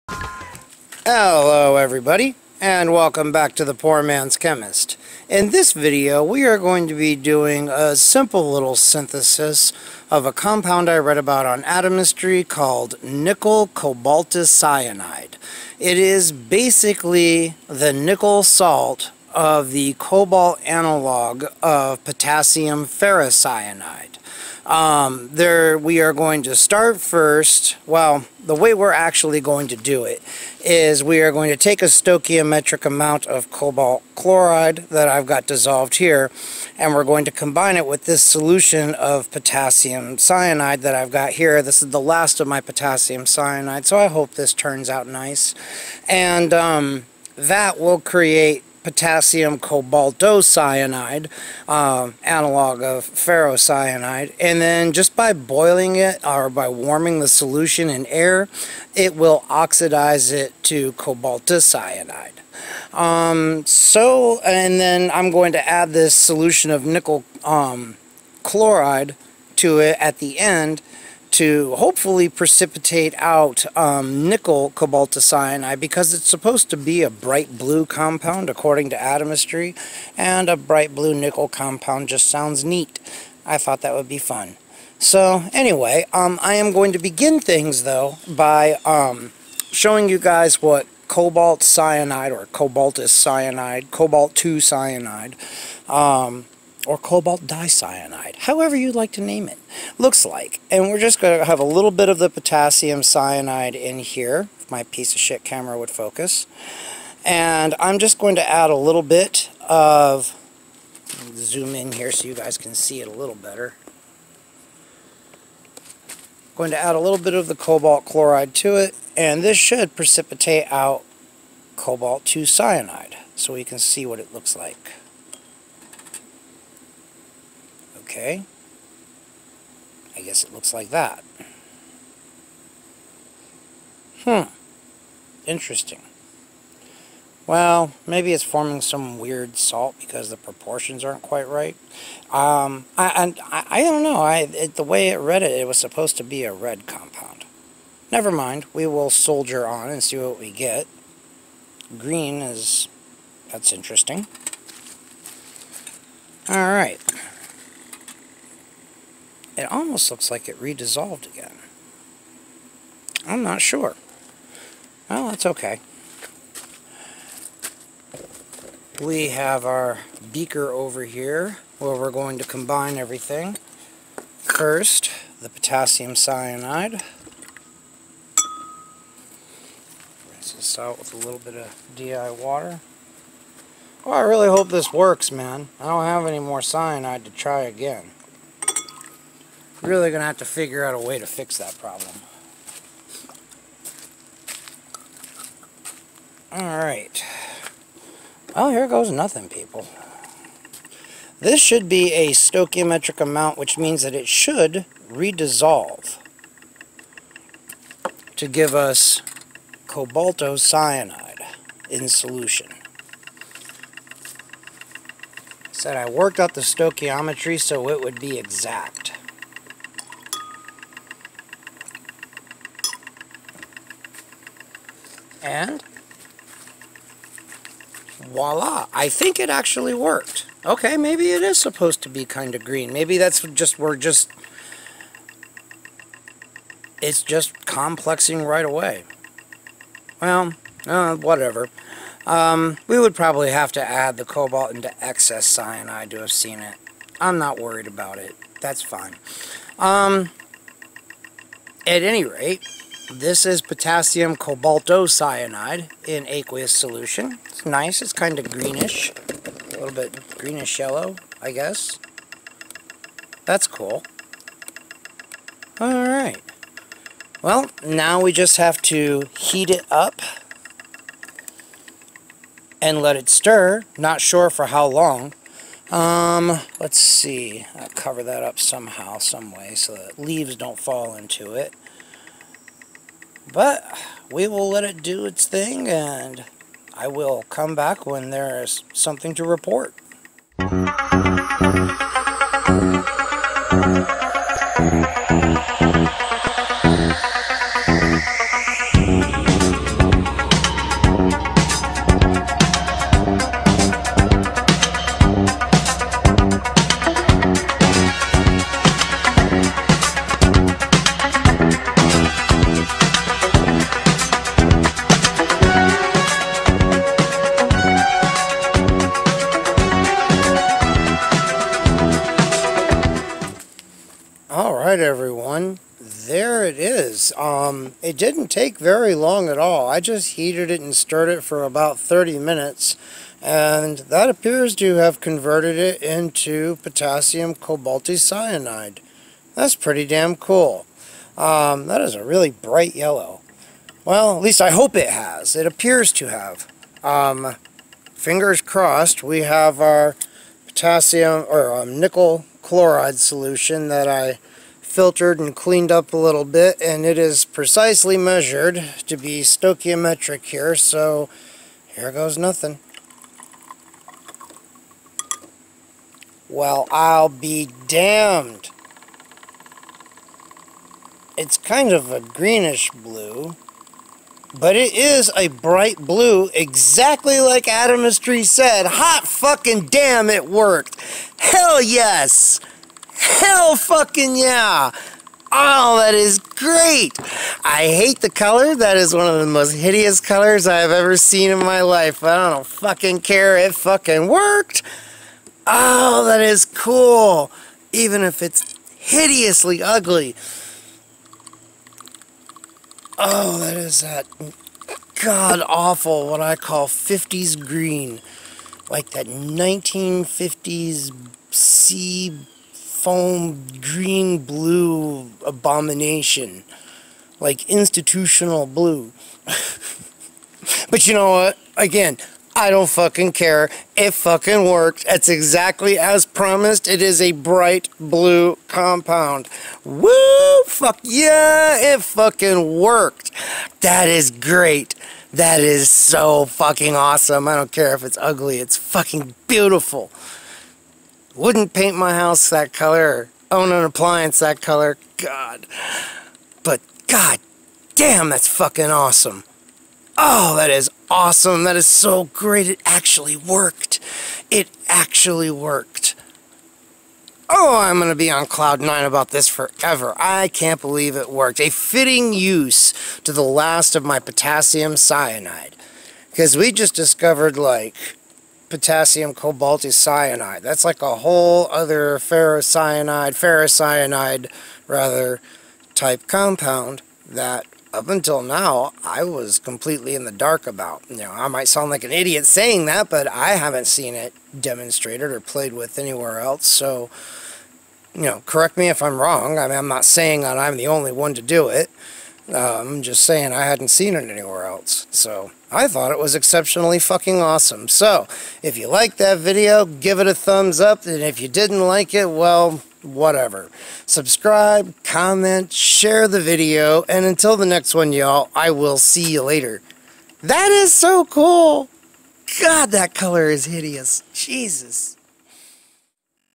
How to Make Potassium Cobalticyanide (Cobalt Analog of Potassium Ferricyanide)
In this video I demonstrate the synthesis of nickel cobalticyanide from potassium cyanide, cobalt chloride, and nickel chloride. Cobalticyanide is the cobalt analog of ferricyanide. Stoichiometric amounts of cobalt(II) chloride and potassium cyanide are combined in aqueous solution to form potassium cobaltocyanide K4[Co(CN)6] which is soluble in water. The solution is heated and stirred for about 30 minutes before the nickel chloride solution was added to precipitate out nickel cobalticyanide.